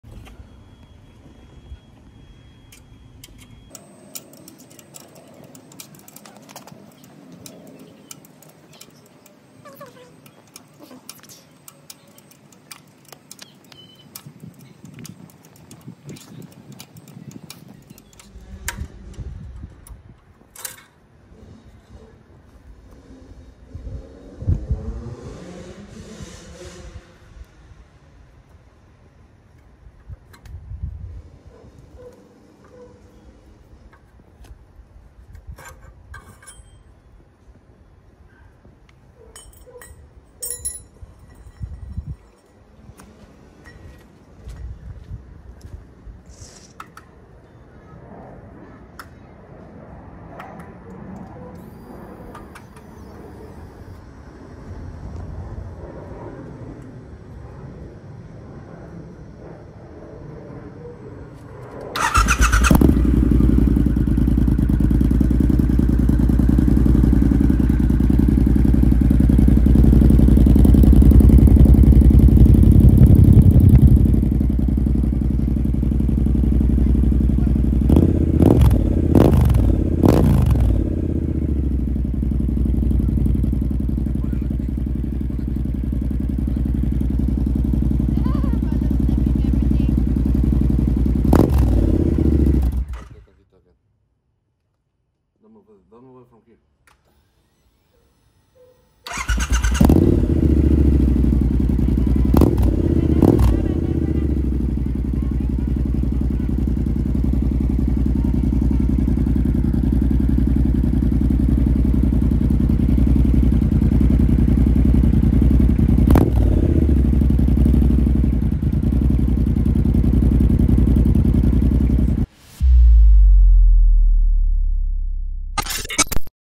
BAFFLE OUT ! I removed the baffle out of my TEC EXHAUST
I decide to remove the baffle out if my triumph scramberXC
and got amazed by the sound of it!!
and all you need is an: allen key